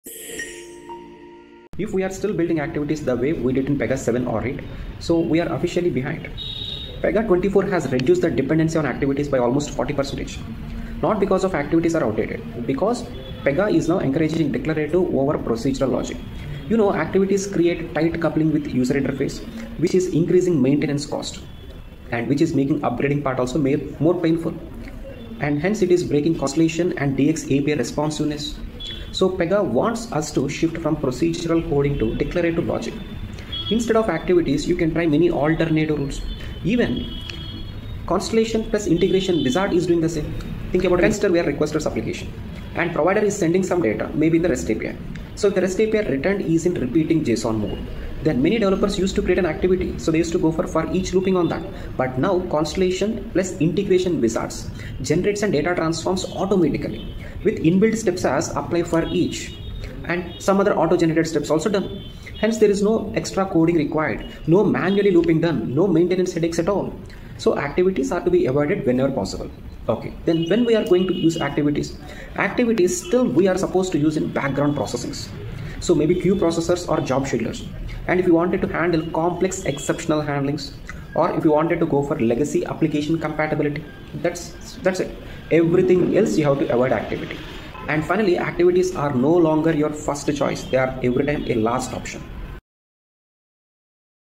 V24 🚫 Activities Are No Longer the First Choice in Pega!
In modern Pega applications, Activities are not the first option anymore.
Best practices recommend using Data Transforms, When Rules, Decision Tables, and Declarative Rules before considering an Activity.

Use Activities only when:

✔ Integration logic needs it
✔ You cannot achieve the requirement with other rule types
✔ There is a legacy process dependency

This short explains why and helps you follow Pega guardrails, best practices, and interview expectations.

👉 Follow for more Pega concepts, polls, and interview shorts!